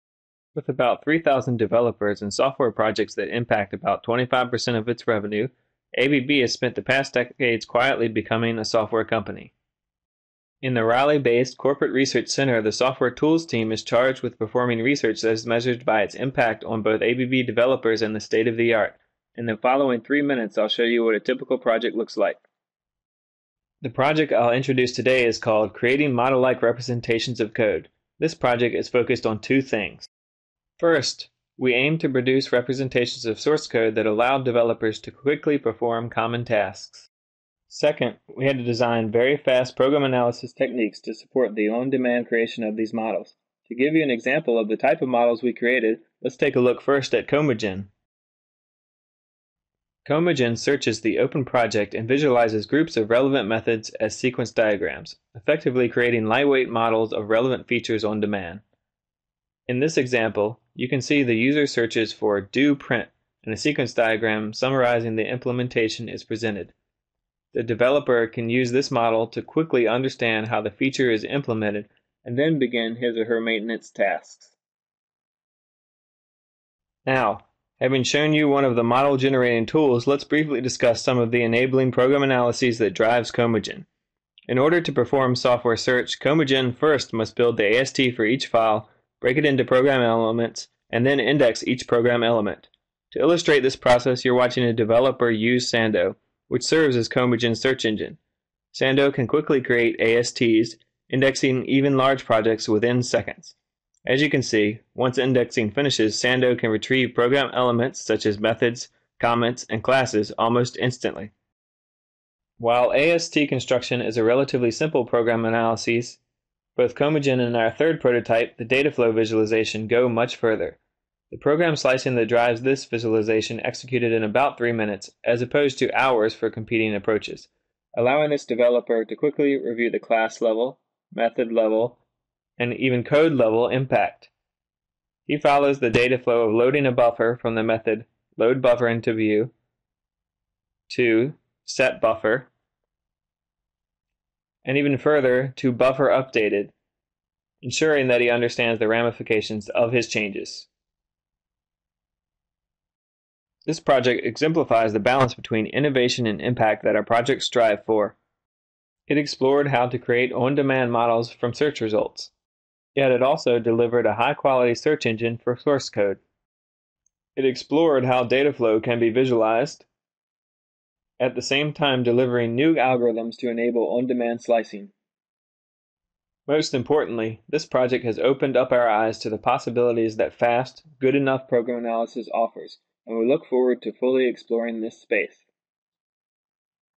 Software Engineering Research at ABB
Here's an overview of a recent project at ABB Software Engineering Lab.